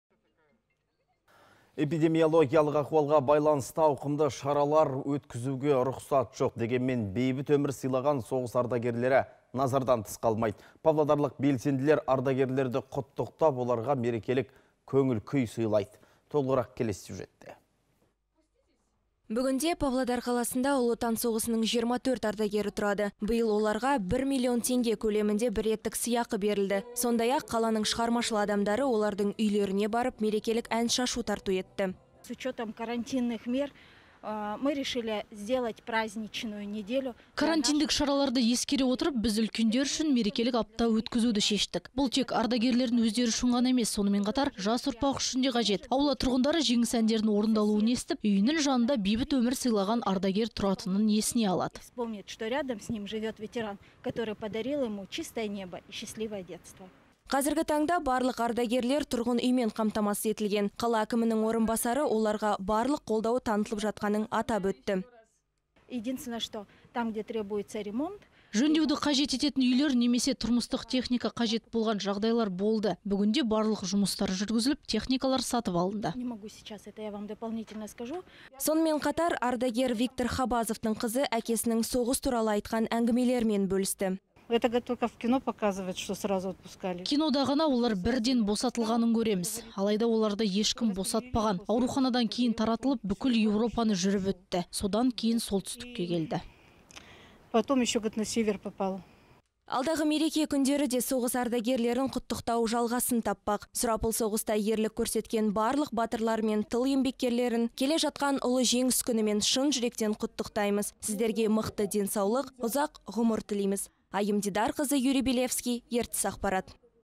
ЕШКІМ ДЕ, ЕШТЕҢЕ ДЕ ҰМЫТЫЛМАЙДЫ
Эпидемиологиялық ахуалға байланысты ауқымды шаралар өткізуге рұқсат жоқ. Дегенмін бейбіт өмір сыйлаған соғыс ардагерлері назардан тыс қалмайды. Павлодарлық белсенділер ардагерлерді құттықтап, оларға мерекелік көңіл-күй сылайды.